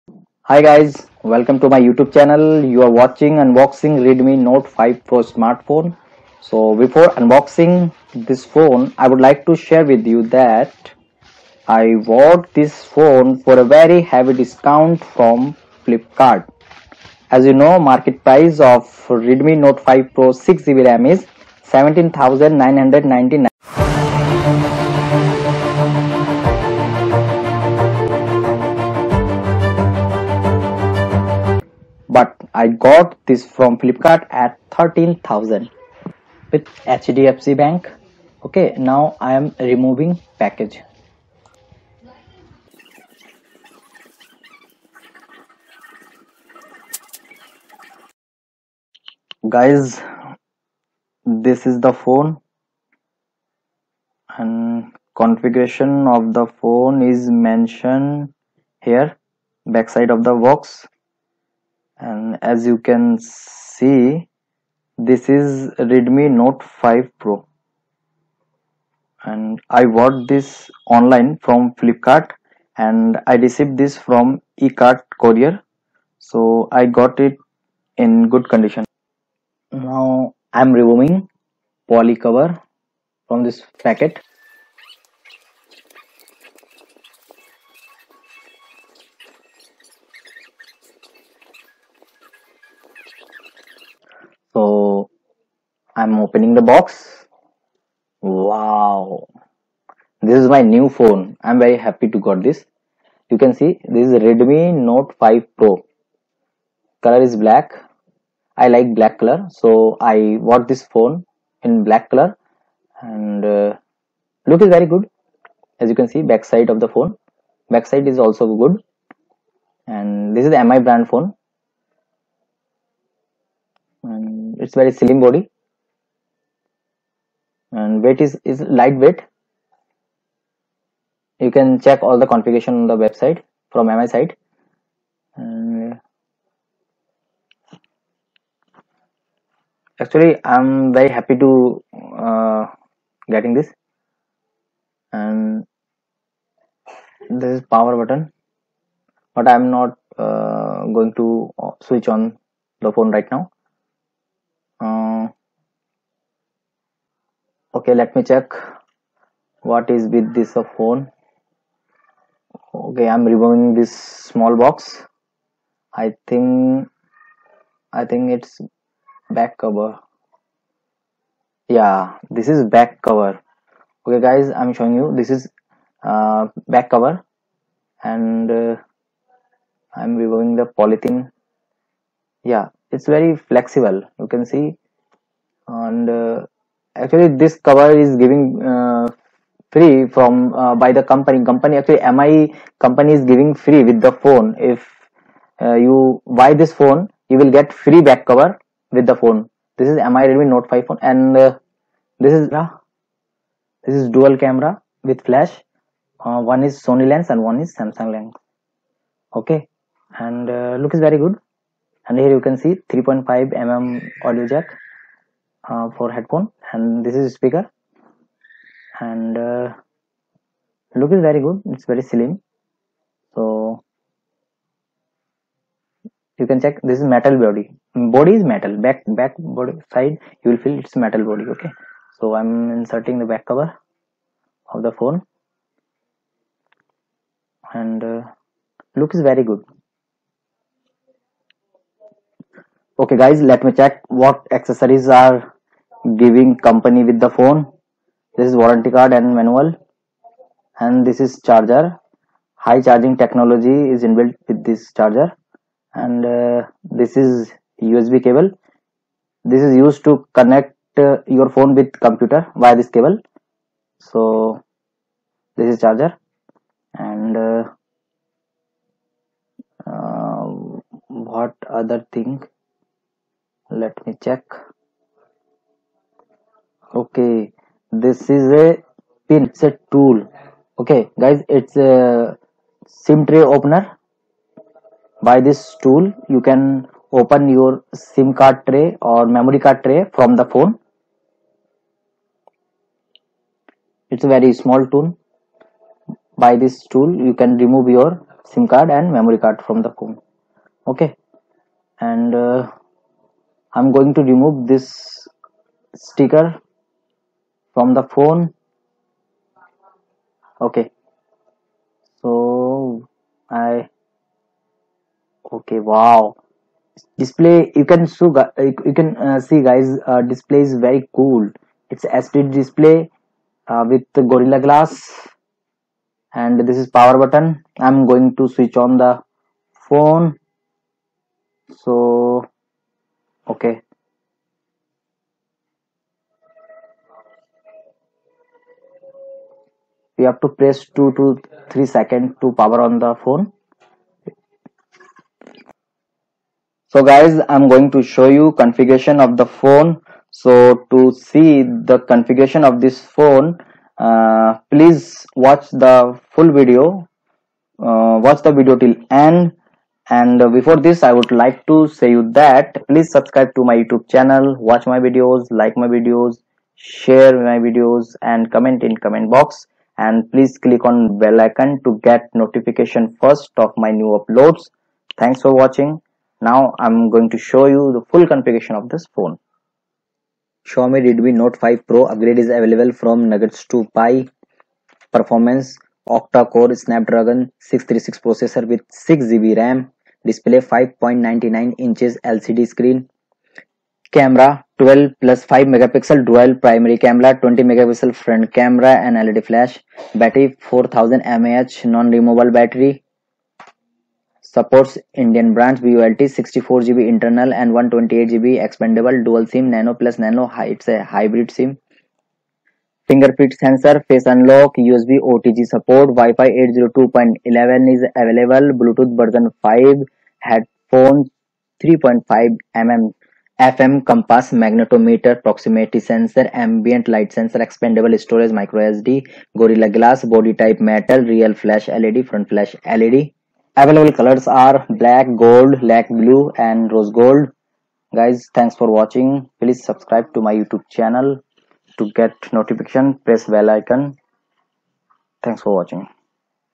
Unboxing and Review of Xiaomi Redmi Note 5 Pro 6 GB RAM
Unboxing & Review of Xiaomi Redmi Note 5 Pro 6 GB RAM | SBIT Online I got this Xiaomi Redmi phone from flipkart @ heavy discount with HDFC Bank Card offer. Unboxing & Feedback of Xiaomi Redmi Note 5 Pro 6 GB RAM सुभाष भक्त द्वारा श्याओमी रेडमी नोट 5 प्रो 6 जीबी रैम की अनबॉक्सिंग और प्रतिक्रिया